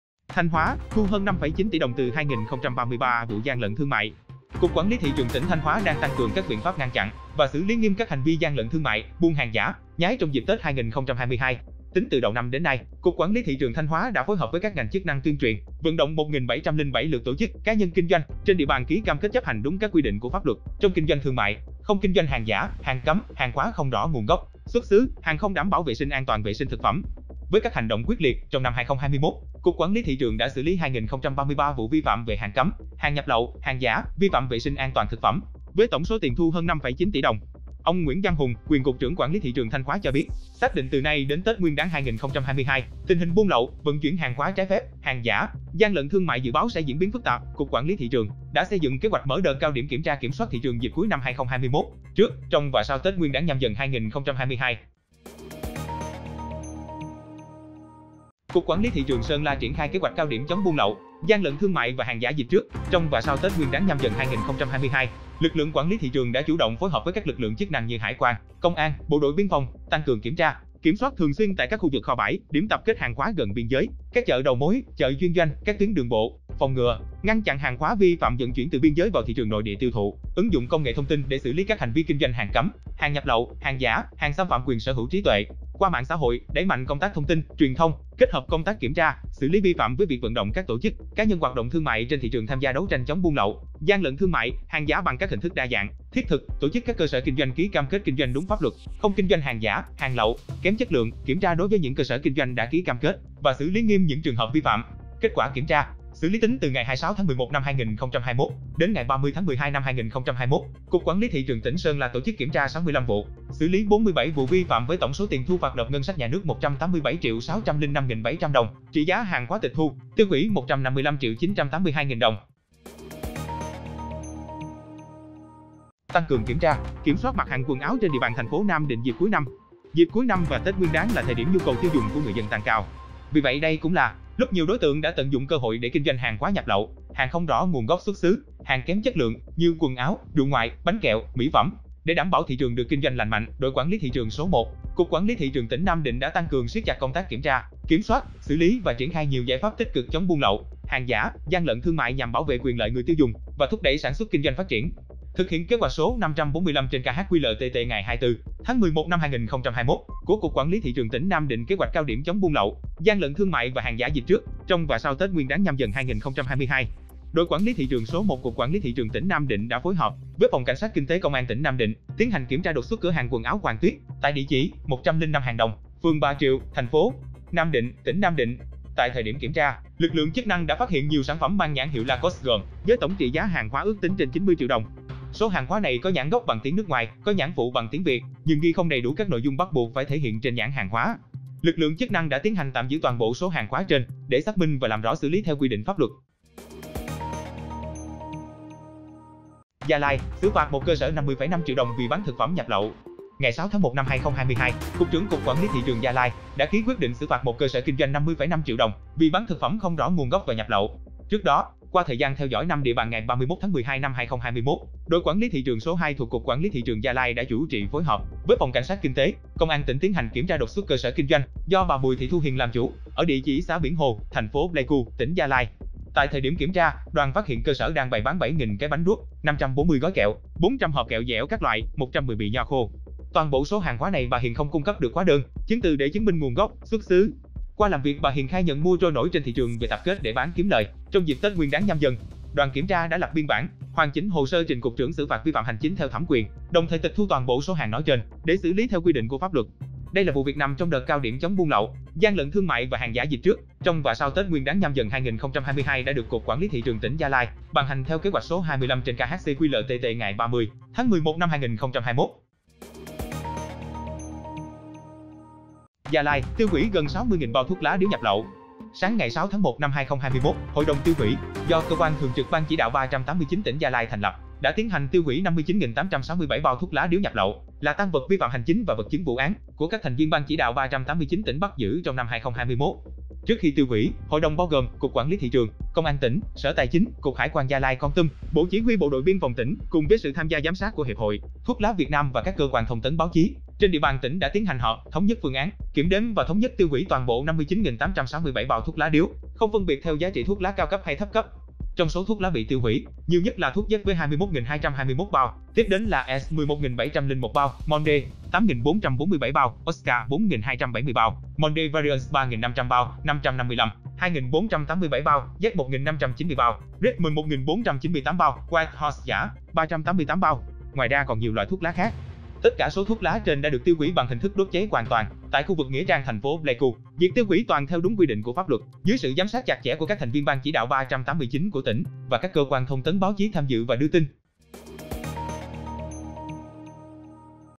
TIN TỔNG HỢP - Thanh Hóa: Thu hơn 5,9 tỷ đồng từ 2.033 vụ gian lận thương mại| Quản lý thị trường
🌟Hãy theo dõi các kênh của TẠP CHÍ QUẢN LÝ THỊ TRƯỜNG để cập nhật nhiều thông tin hữu ích: